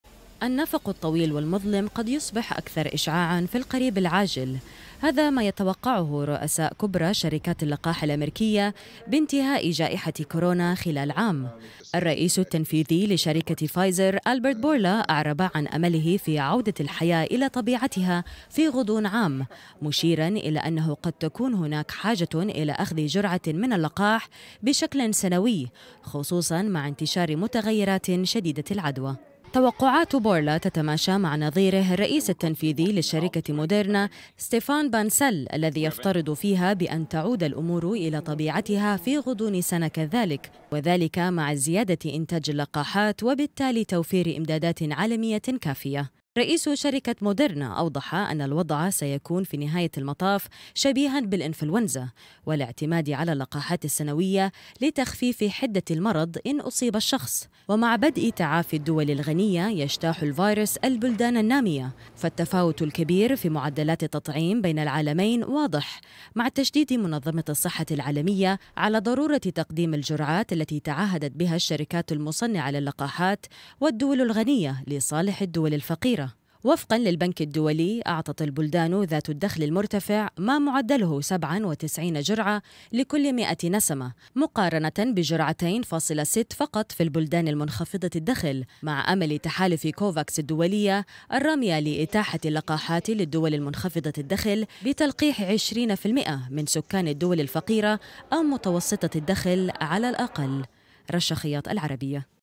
رئيس شركة فايزر يتوقع موعد نهاية كورونا
رئيس شركة فايزر يزف البشرى.. ويتوقع موعد انتهاء جائحة كورونا